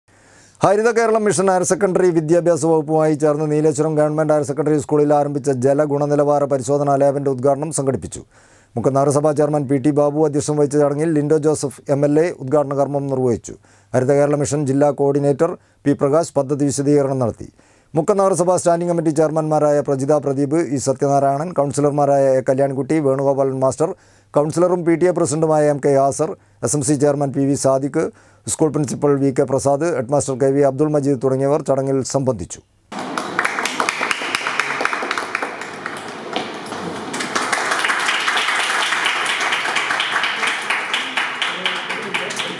ജലഗുണനിലവാര പരിശോധന ലാബിന്റെ ഉത്ഘാടനം സംഘടിപ്പിച്ചു
ഹരിത കേരളം മിഷൻ - ഹയർ സെക്കണ്ടറി വിദ്യാഭ്യാസ വകുപ്പുമായി ചേർന്ന് നീലേശ്വരം ഗവർമെന്റ് ഹയർ സെക്കണ്ടറി  സ്കൂളിൽ ആരംഭിച്ച ജലഗുണനിലവാര പരിശോധന ലാബിന്റെ ഉത്ഘാടനം സംഘടിപ്പിച്ചു.